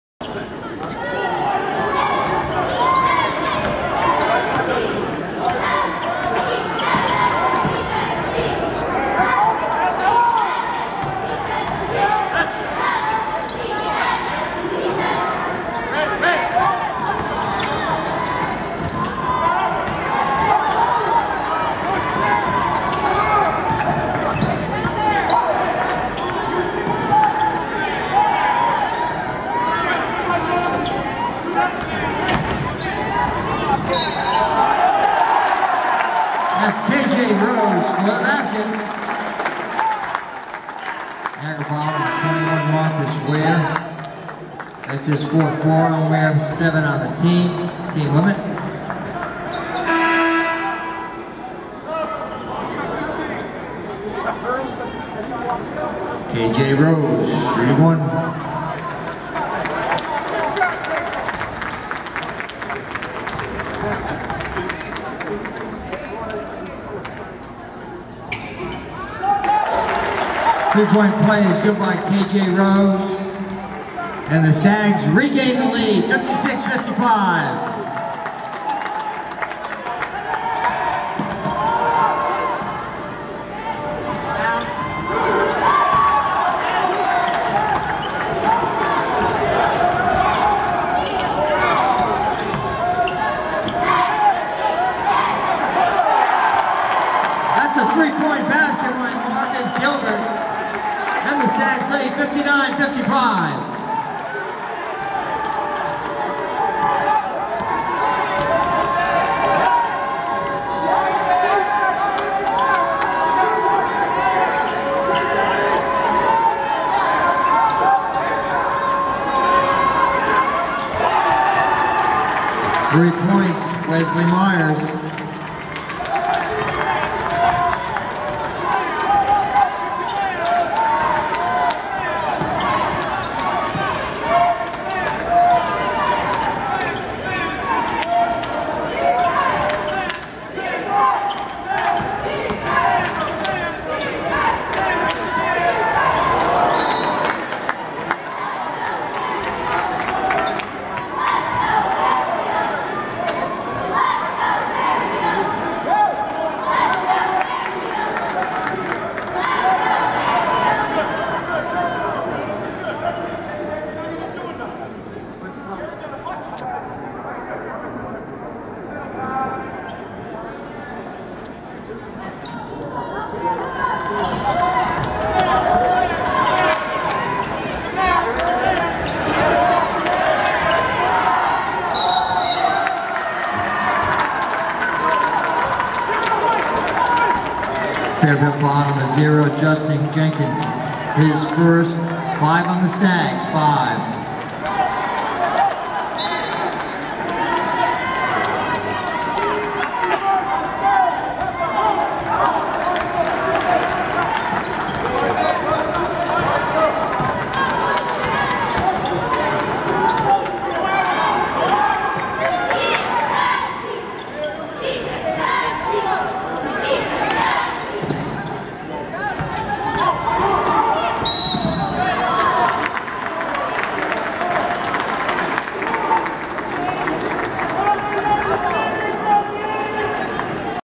Niagara vs Fairfield - Men's Basketball - 2nd Half - Gilbert and Myers 3-pointers - Jan 16, 2014
Niagara Purple Eagles vs Fairfield Stags
Men's Basketball  -  2nd Half Action
K.J. Rose drive to the hoop scores and is fouled  -  3-point play
Marcus Gilbert 3-pointer
Wesley Myers 3-pointer
Justin Jenkins commits offensive foul
Webster Bank Arena  -  Bridgeport, Connecticut
Thursday January 16, 2014Niagara Purple Eagles vs Fairfield Stags